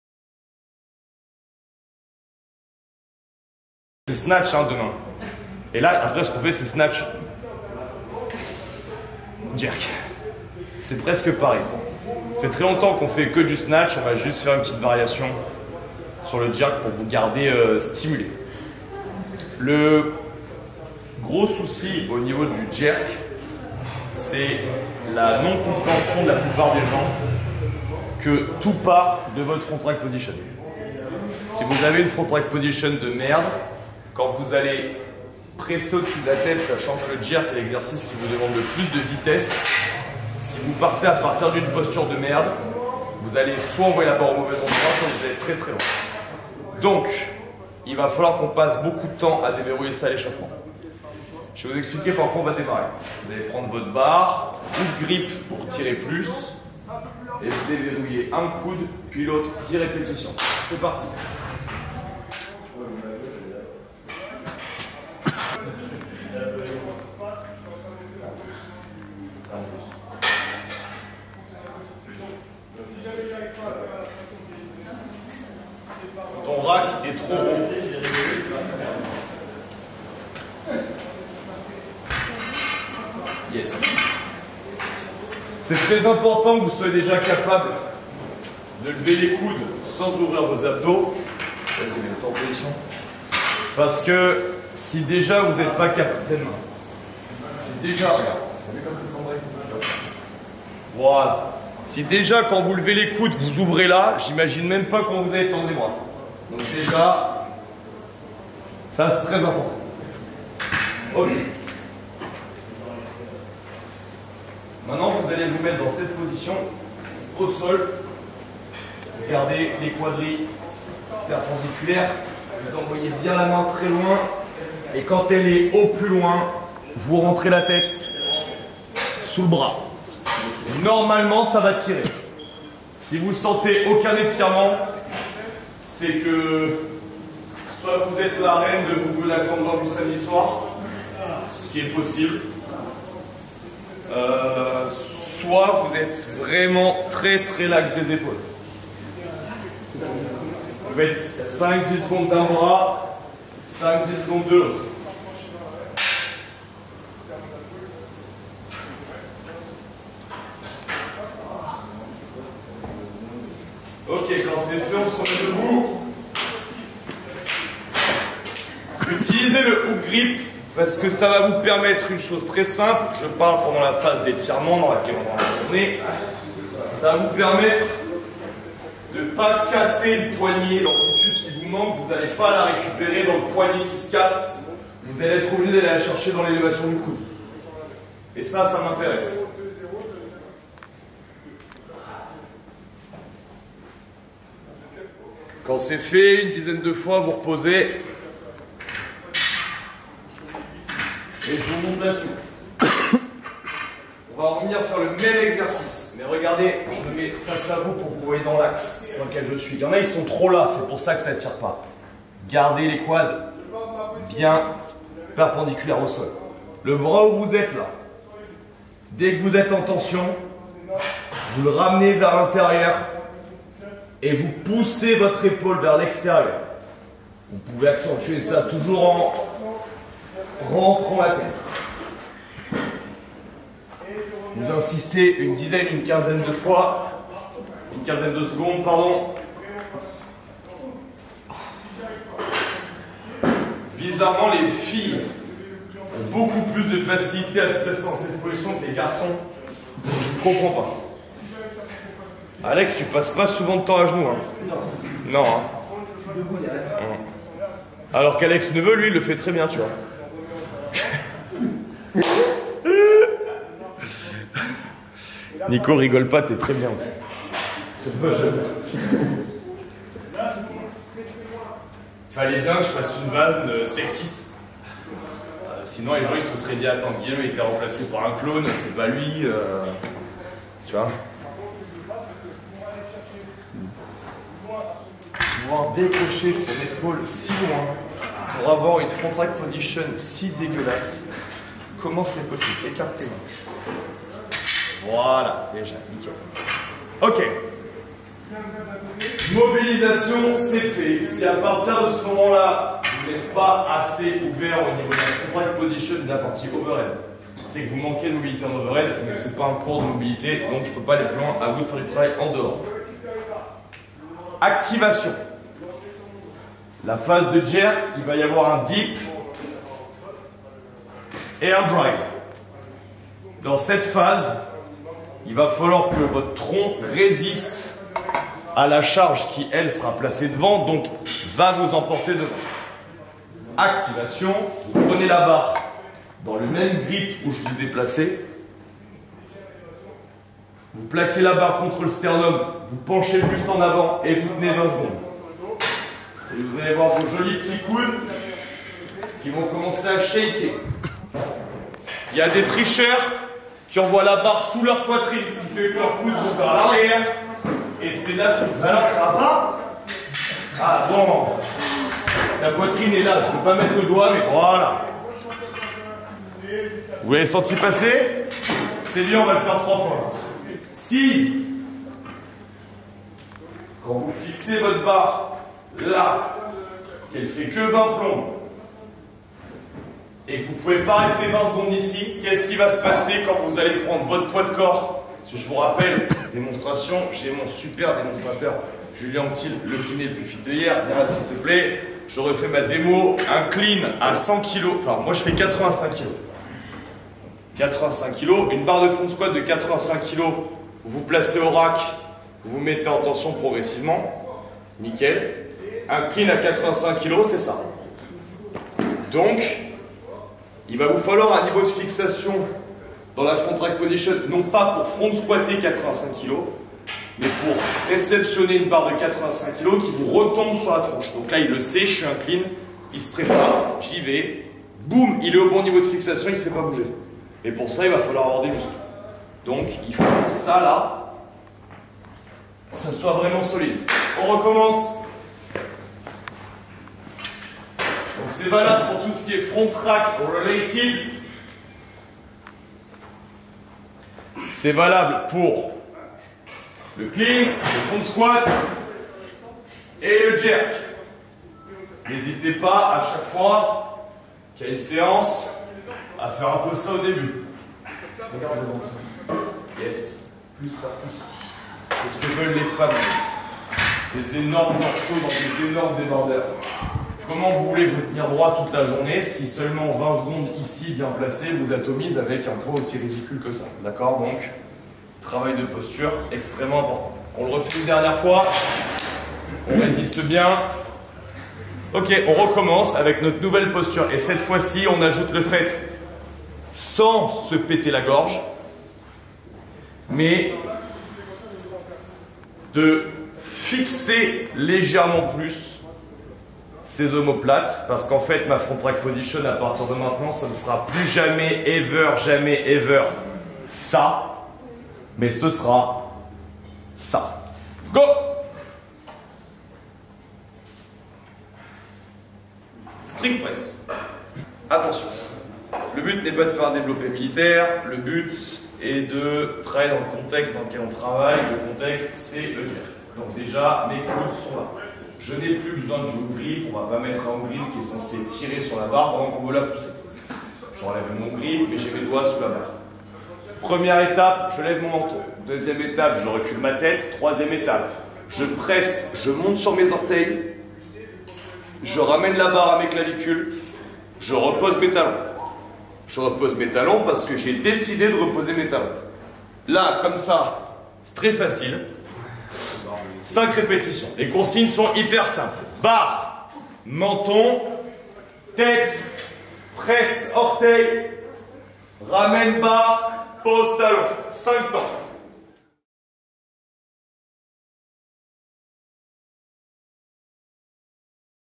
Jerk Activation
Aujourd'hui, je t'offre l'extrait d'un cours d'haltérophilie à mes côtés.

✅ Activation pour le jerk : résister à l'effet de la charge sur votre posture
✅ Déverrouillage du Front rack
✅ Posture et mobilité

Tu peux te servir des différents exercices proposés dans cette vidéo pour améliorer tes performances et ta mobilité sur le jerk.
Si tu en veux encore plus, tu peux opter pour nos programmations made in Waath 👉🏻